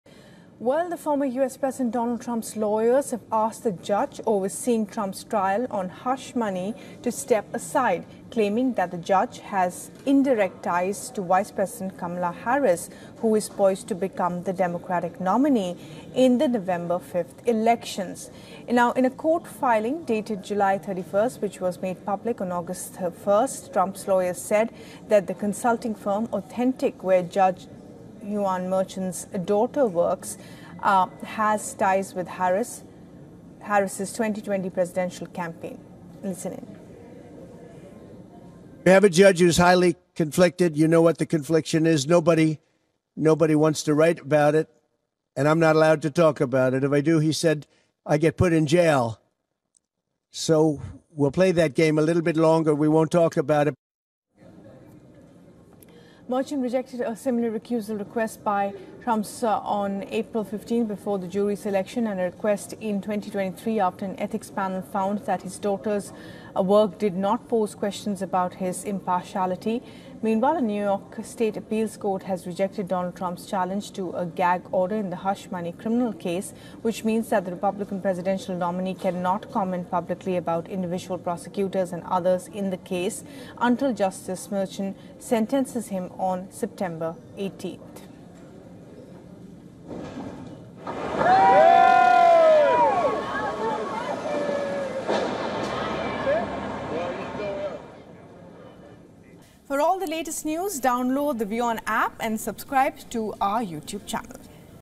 Trump lawyers seek recusal of judge in hush-money case over daughter’s work with Harris | WION
Donald Trump's lawyers again asked the judge overseeing the former U.S. president's criminal trial on charges stemming from hush money paid to a porn star to recuse himself, citing his daughter's work for Vice President Kamala Harris.